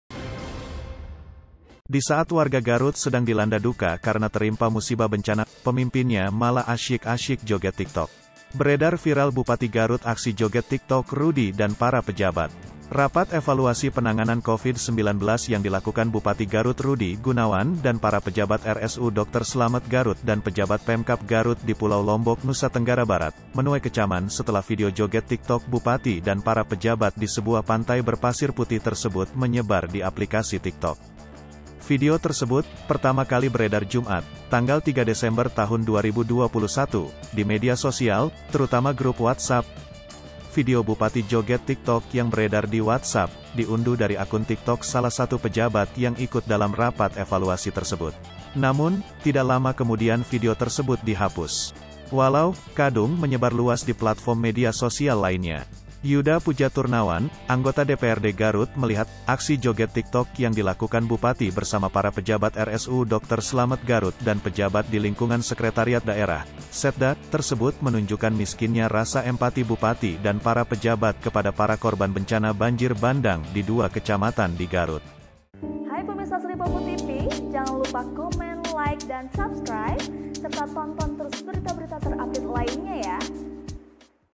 Disaat Garut Sedang Dilanda Banjir, Bupati Garut Asyik Joget TikTok
Disaat warga Garut sedang dilanda duka karena terimpa musibah bencana, pemimpinnya malah asyik-asyik joget TikTok.

Beredar viral Bupati Garut aksi joget tiktok Rudy dan para pejabat.